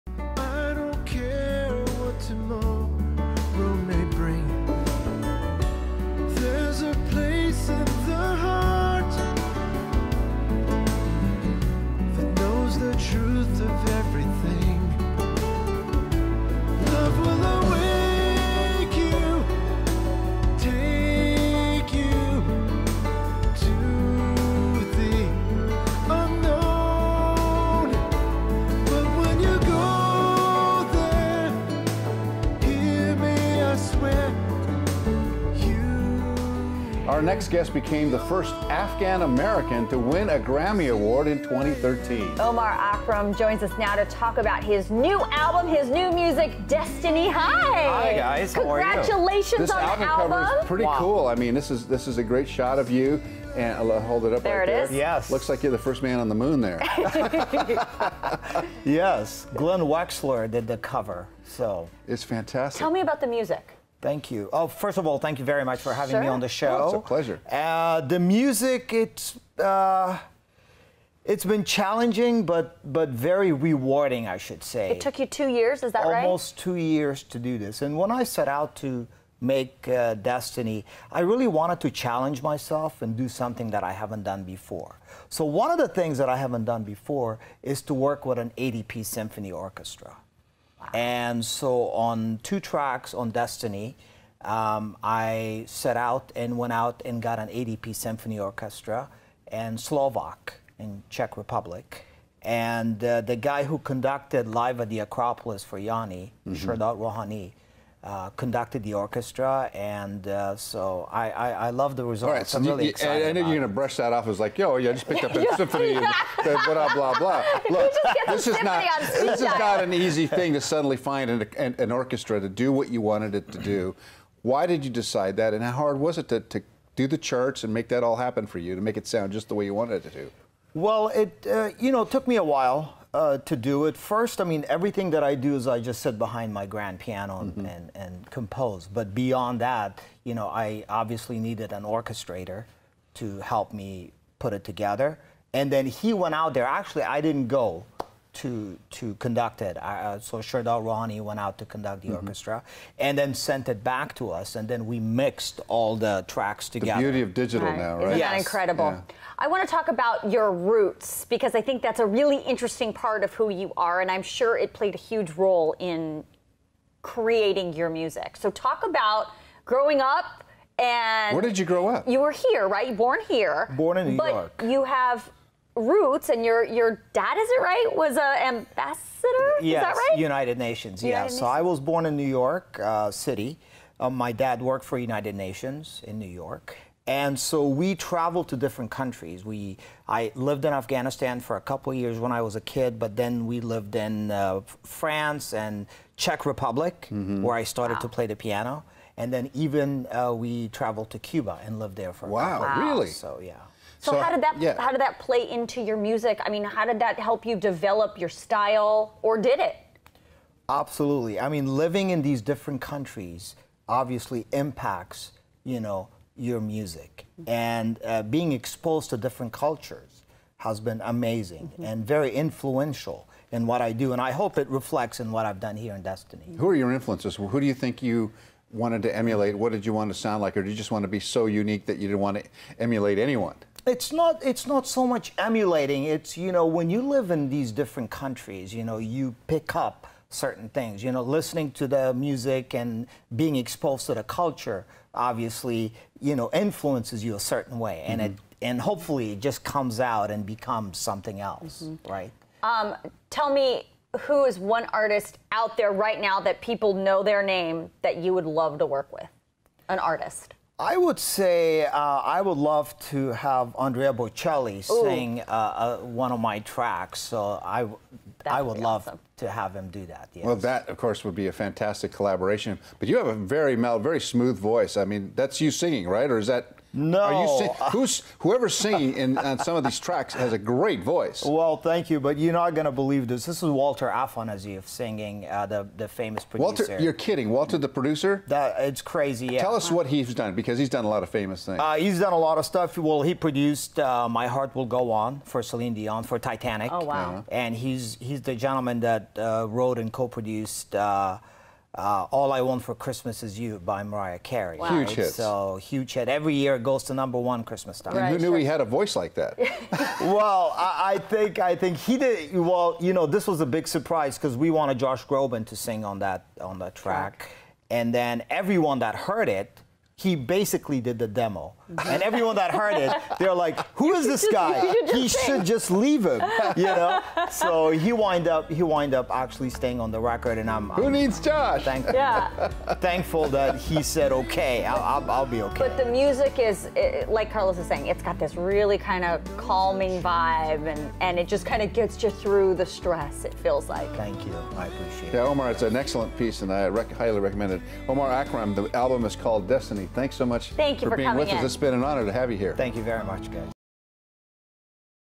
Musician Omar Akram talks about his latest album
Carlos & Lisa talk to Grammy Award-winner Omar Akram about his new music.